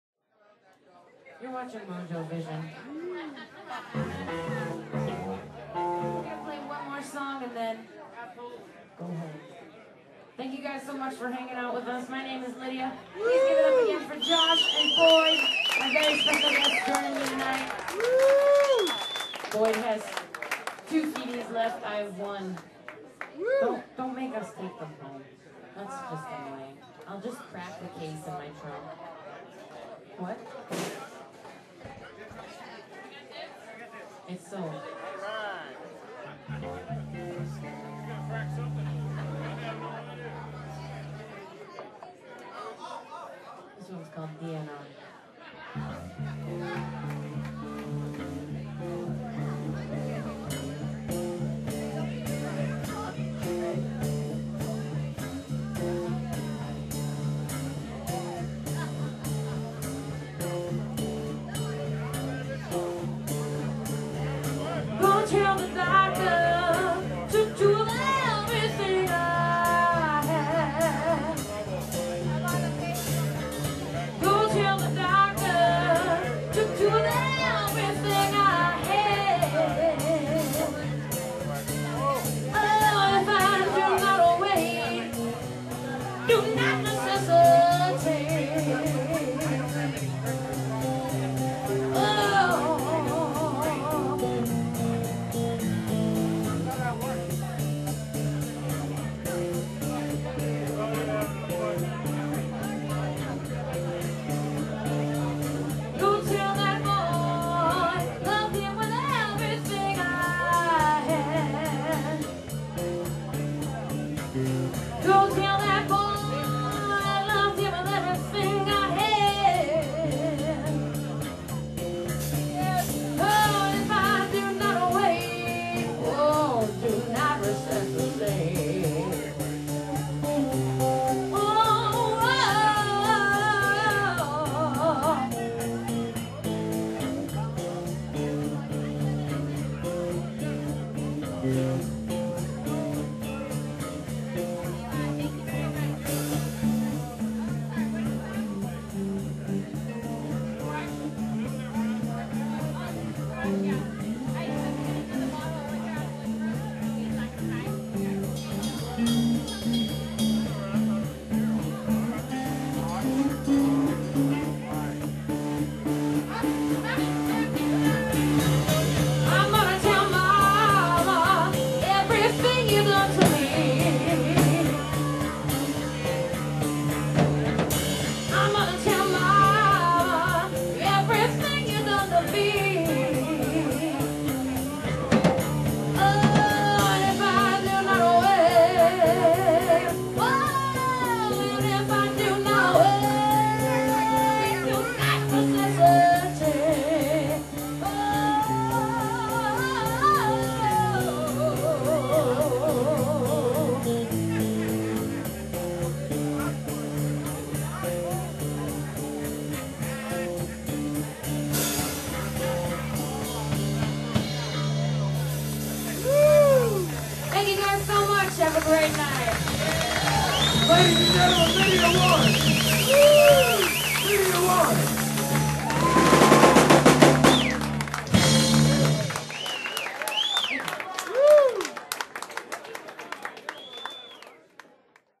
The Lydia Warren Band Live @ The Back Page 6/23/12
Lots of Friends, Lots of Fun @ The Back Page with The Lydia Warren Band and Special Guest Boyd Small : ) Great line-up with Lydia Warren- guitar/vocals, JJ "Guitar Face" Fulero-bass/vocals, and Boyd Small-drums/vocals!